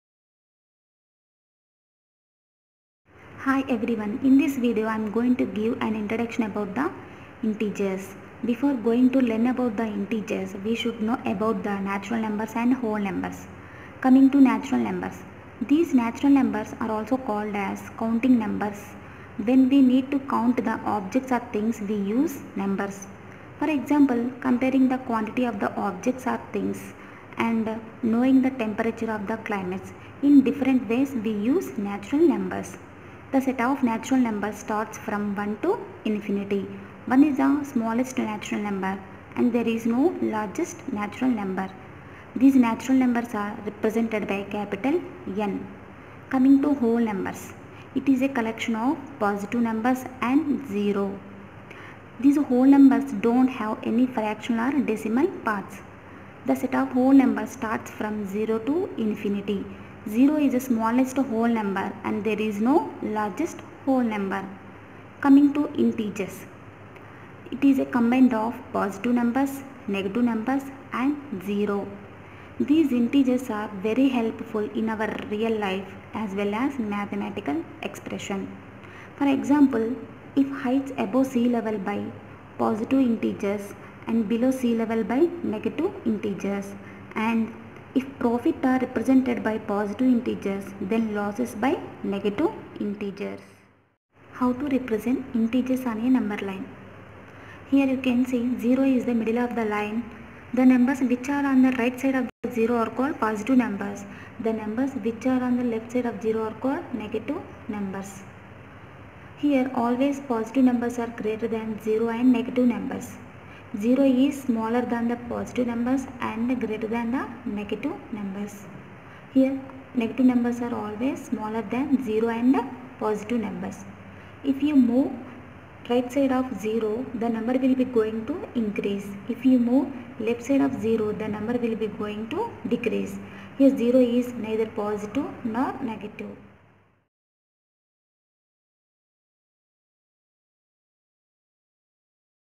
Introduction about the integers//Chapter-6//Class -6//part-1.
In this video I clearly explained about an introduction of integers.In the next video I will upload remaining topics with solutions of integers lesson.